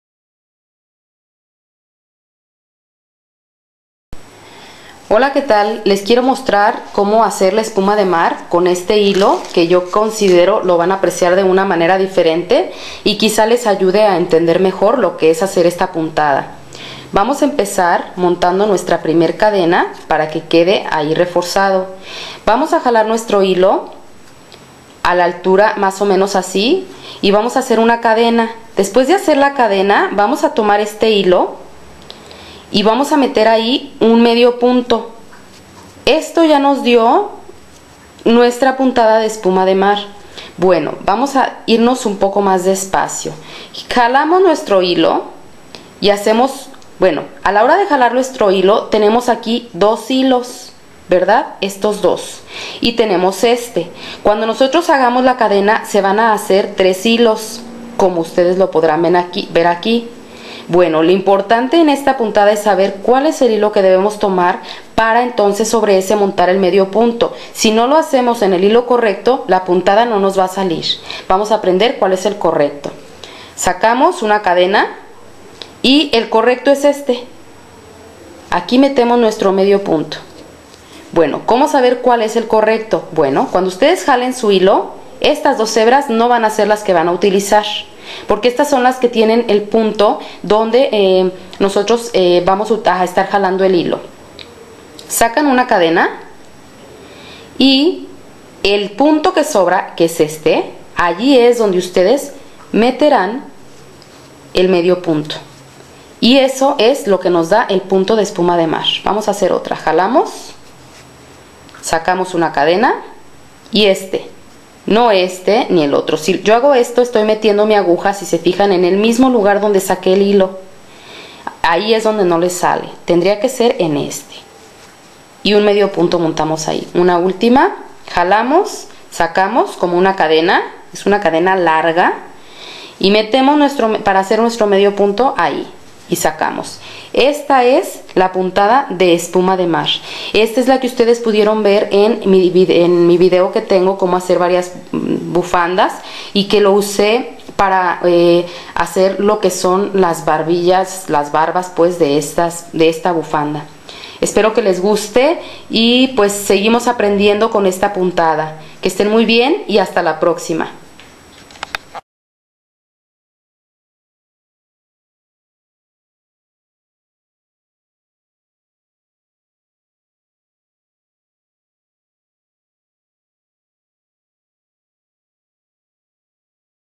G36 COMO TEJER ESPUMA DE MAR SIMPLE, SALOMON, MARIPOSA GANCHILLO CROCHET
AQUI LES MUESTRO LA PUNTADA ESPUMA DE MAR QUE TAMBIEN LLAMAN PUNTO SALOMON, EN UN HILO QUE PODRAN APRECIAR MEJOR LA PUNTADA. PUEDEN HACER CHALINAS, CHAL, CAPA, ETC.